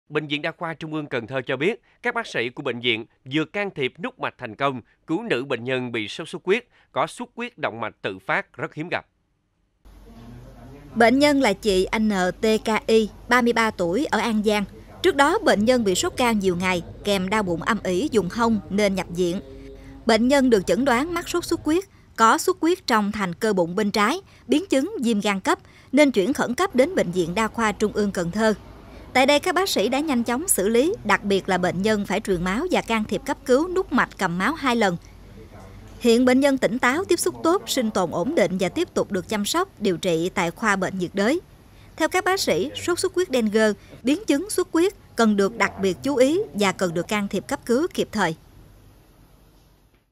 Cứu bệnh nhân bị biến chứng do sốt xuất huyết
Trên lĩnh vực y tế, Bệnh viện Đa khoa Trung ương Cần Thơ cho biết, các bác sĩ của bệnh viện vừa can thiệp nút mạch thành công cứu nữ bệnh nhân bị sốt xuất huyết có xuất huyết động mạch tự phát rất hiếm gặp.